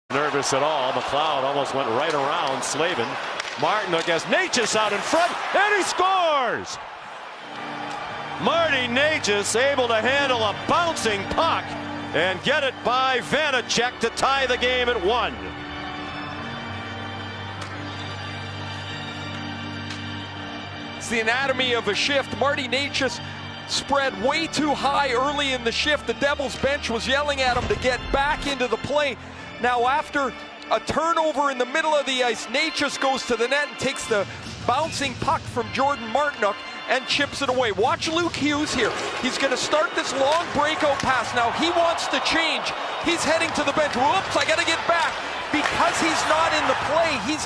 Martin Nečas nice goal vs Devils in game 4 (5/9/23)
Nečasův gól ve čtvrtém utkání série mezi New Jersey a Carolinou.
Czech superstar Martin Necas scores with great hand-eye coordination against the New Jersey Devils in game 4.